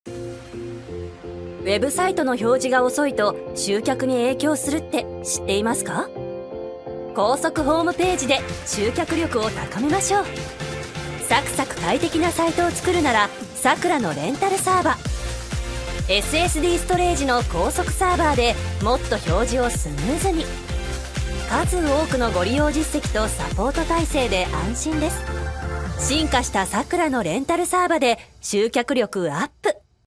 高速Webサイトで集客力UP！快適なサイトを作るなら「さくらのレンタルサーバ」
Webサイトの表示速度が遅いと集客にも影響するってご存知でしたか？
SSDストレージの高速サーバーを採用したさくらのレンタルサーバなら、サクサク快適に表示されるので、ストレスフリー＆SEO評価アップ。高速ホームページで集客力を高めましょう！

さくらのレンタルサーバはこちらから